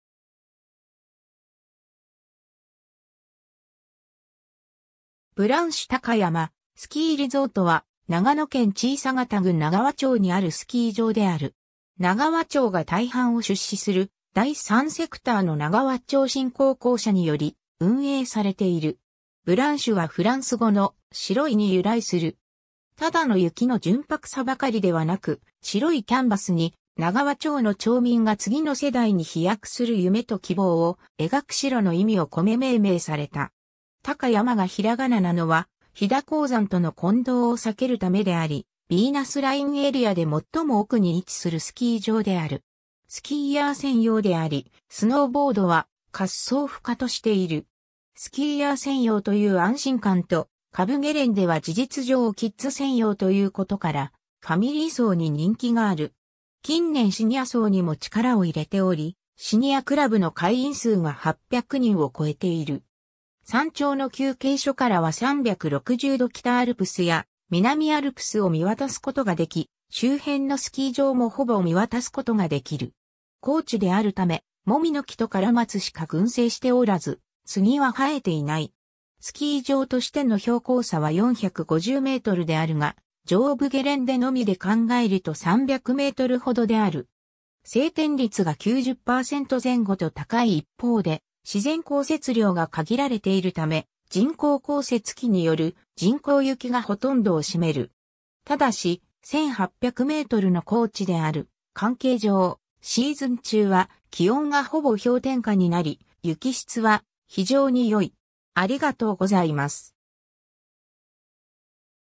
ブランシュたかやまスキーリゾート
ブランシュたかやまスキーリゾート（ぶらんしゅたかやますきーりぞーと）は、長野県小県郡長和町にあるスキー場である。
長和町が大半を出資する第三セクターの長和町振興公社により運営されている。
 ブランシュはフランス語の“白い”に由来する。
ただの雪の純白さばかりではなく、白いキャンバスに長和町の町民が次の世代に飛躍する夢と希望を描く白の意味を込め命名された。
たかやまがひらがななのは、飛騨高山との混同を避けるため（実際には鷹山）であり、ビーナスラインエリアで最も奥（北）に位置するスキー場である。
 スキーヤー専用であり、スノーボードは滑走不可としている。
スキーヤー専用という安心感と、下部ゲレンデは事実上キッズ専用ということから、ファミリー層に人気がある。
近年シニア層にも力を入れており、シニア倶楽部の会員数が800人を超えている（2017年）。
 山頂の休憩所からは360度北アルプスや南アルプスを見渡すことが出来（富士山も見ることが出来る）、周辺のスキー場もほぼ見渡すことが出来る。
高地であるため、モミの木とカラマツしか群生しておらず、杉は生えていない。
スキー場としての標高差は450mであるが、上部ゲレンデのみで考えると300mほどである。
晴天率が90％前後と高い一方で、自然降雪量が限られているため、人工降雪機による人工雪がほとんどを占める。
ただし、1800mの高地である関係上、シーズン中は気温がほぼ氷点下になり、雪質は非常によい。